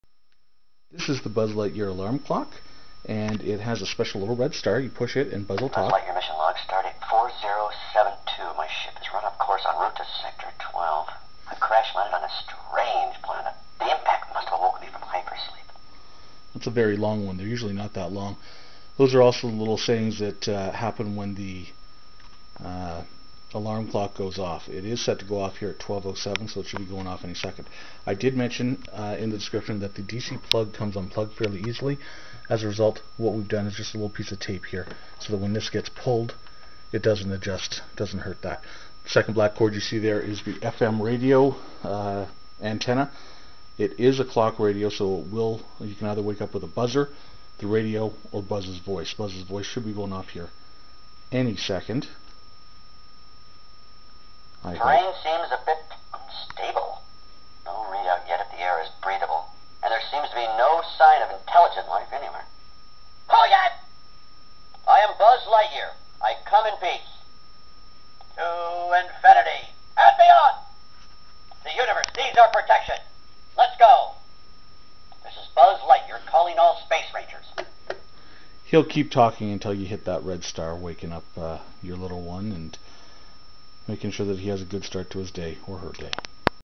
Buzz Lightyear Talking Digital Alarm Clock
What the Buzz Lightyear alarm clock sounds like and how to use it.